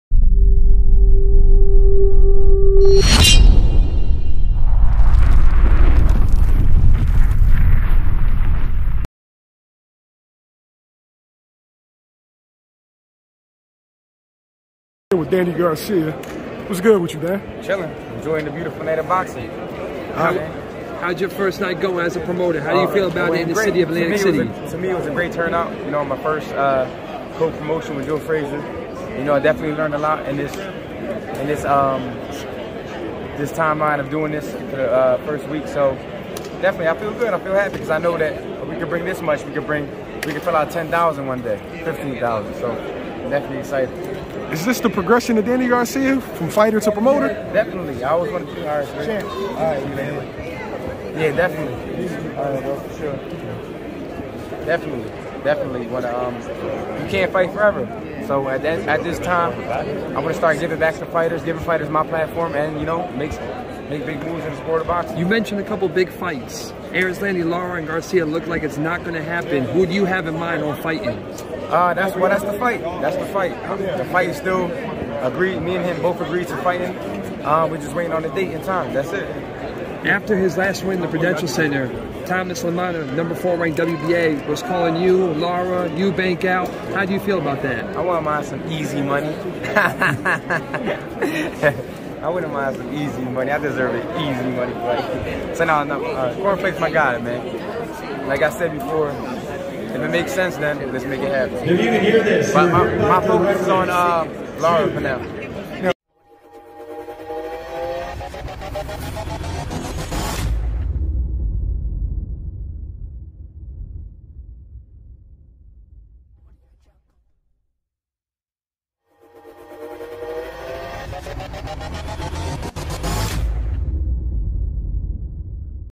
"EASY MONEY" Danny Garcia talks Erislandy Lara, Thomas Lamanna & becoming a Promoter
Former 2 Division World Champion Danny Garcia 37-3 21KO's touches on transitioning to become promoter, moving to Middleweight to face Erislandy Lara & being called out by Thomas Lamanna.